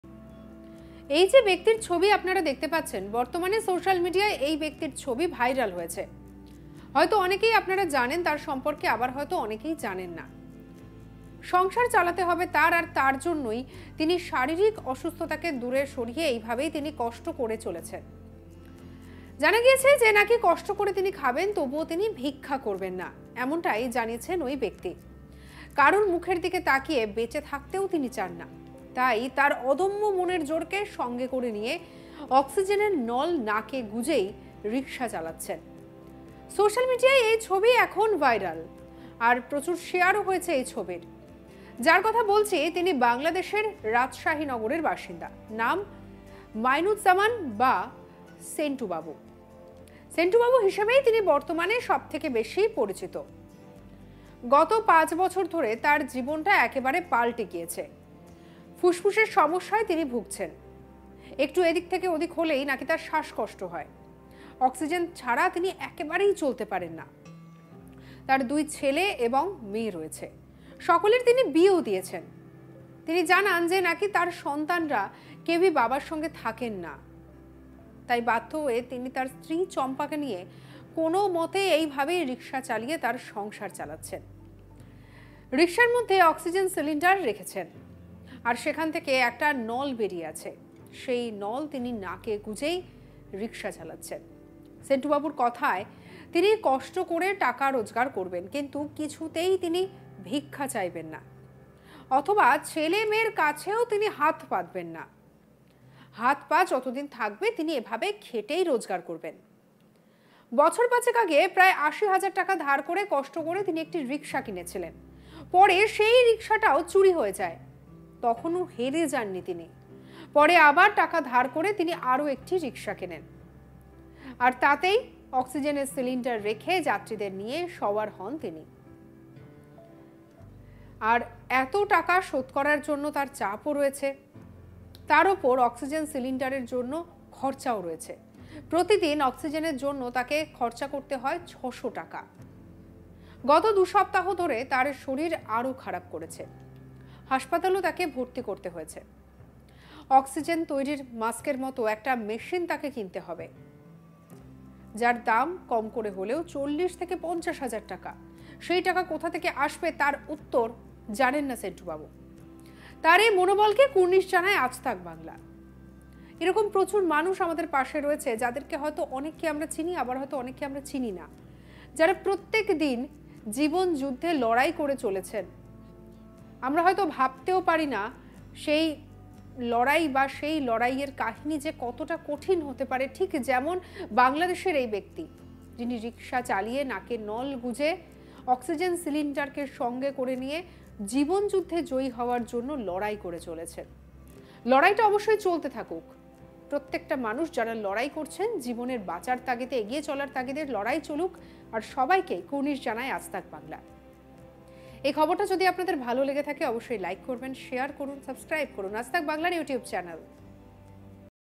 ​'কষ্ট করে খেটে খাব, তবু ভিক্ষা করব না', নাকে অক্সিজেন নল গুঁজে রিক্সা টানেন সেনটু বাবু| Rickshaw
সংসার চালাতে হবে। কিন্তু শারীরিক অসুস্থতার জন্য কাজ করতে কষ্ট হয় তাঁর। কিন্তু না, তাও তিনি ভিক্ষা করবেন না।  কারও মুখের দিকে তাকিয়ে বেঁচে থাকতেও তিনি চান না। তাই অক্সিজেন নল নাকে লাগিয়েই রিক্সা চালাচ্ছেন তিনি। সোশ্যাল মিডিয়ায় তাঁর এই ছবি এখন ভাইরাল। যাঁর কথা বলছি, তিনি বাংলাদেশের রাজশাহী নগরের বাসিন্দা। নাম মাইনুজ্জামান হলেও তিনি সেন্টুবাবু হিসাবেই পরিচিত। গত 5 বছর ধরে তাঁর জীবনটা একেবারে পাল্টে গেছে। ফুসফুসের সমস্যায় ভুগছেন তিনি। একটু এদিক ওদিক করলেই শ্বাসকষ্ট হয় তাঁর।

Bangladeshi Rickshaw puller riding with oxygen cylinder due to the problem of lungs